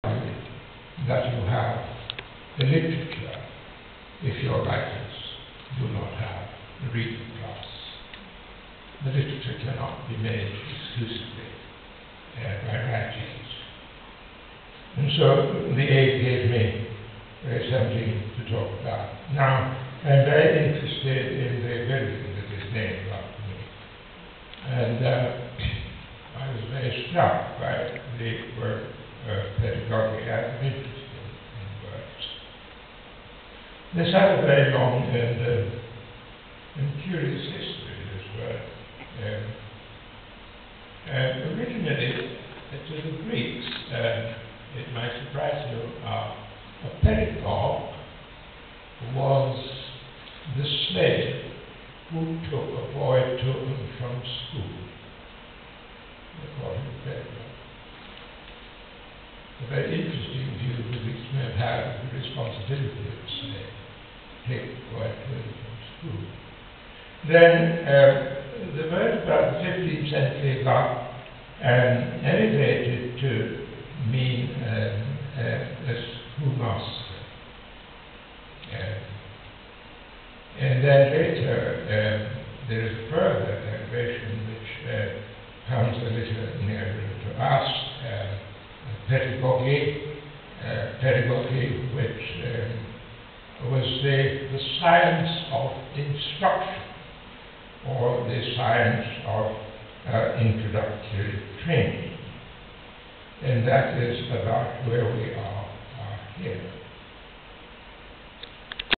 Barbadian Author George Lamming on evolution of Language: Padagogical Institute #1
Author of "Castle Of My Skin" looks at origin of key word in the building for which he's to be named with, "The George Lamming PEDAGOGICAL Institute" spoke to a packed audience at UWI on 23/6/2009